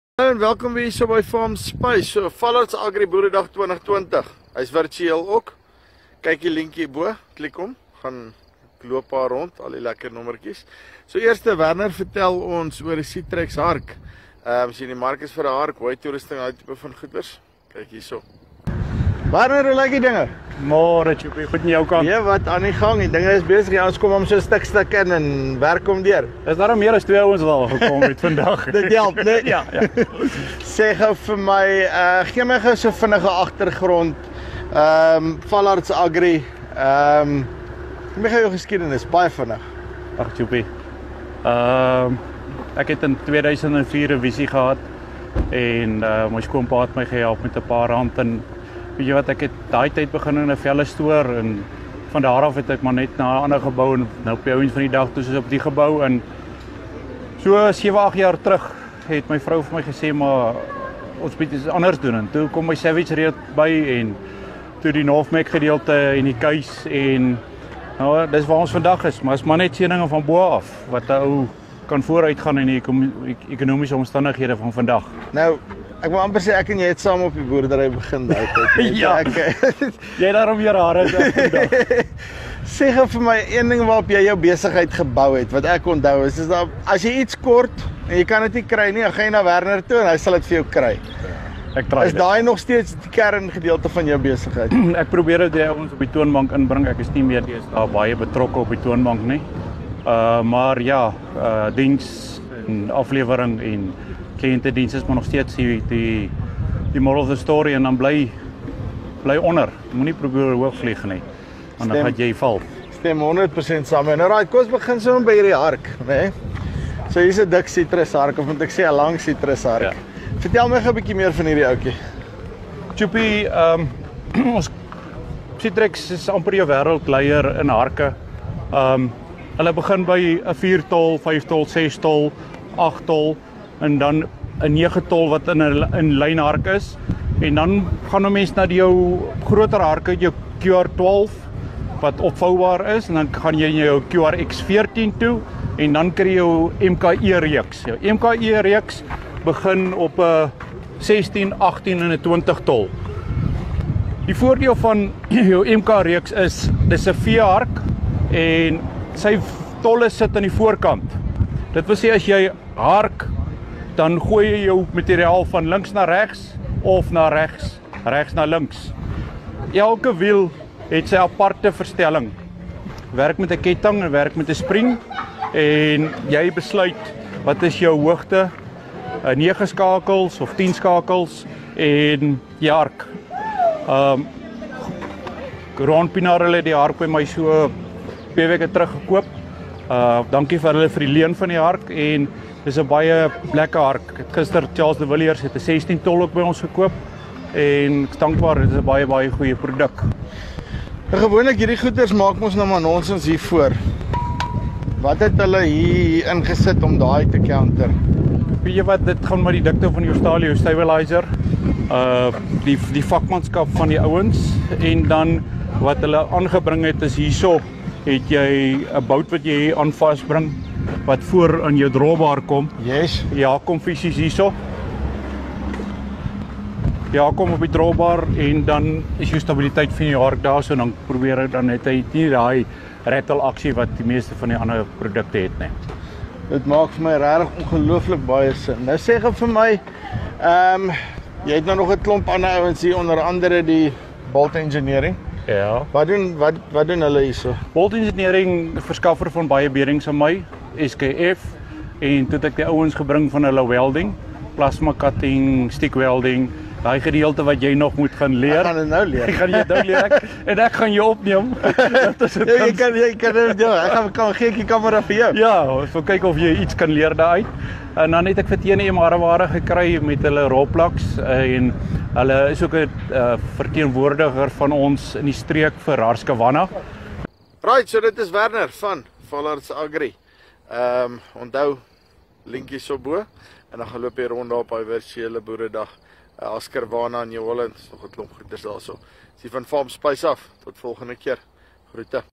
Vaalharts Agri Boeredag 2020 - Sitrex Hark
Virteel Soek jy 'n WYE hark? Werner vertel meer oor hierdie massiewe Sitrex hark. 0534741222